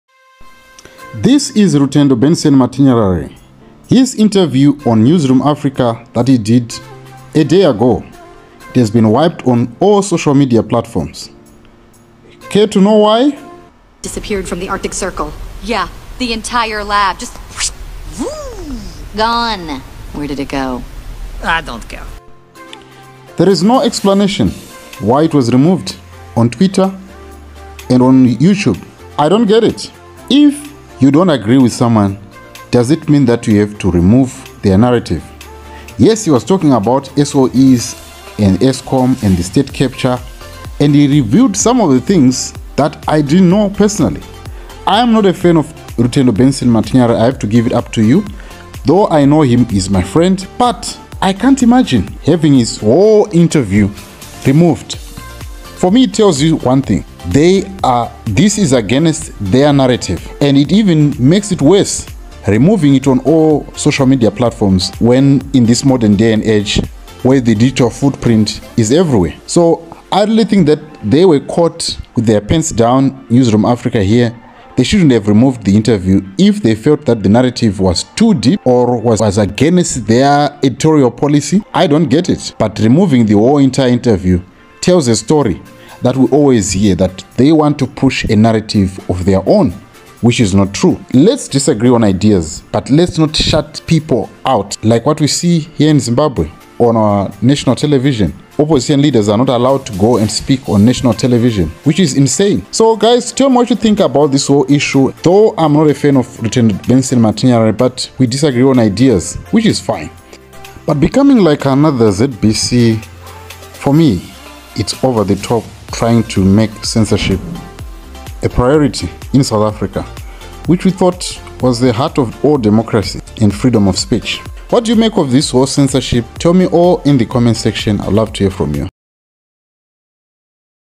MUST WATCH|Why Rutendo Matinyarare was censored on Newsroom Africa
The anti sanctions March activist Rutendo Matinyarare Interview removed by Newsroom Afrika.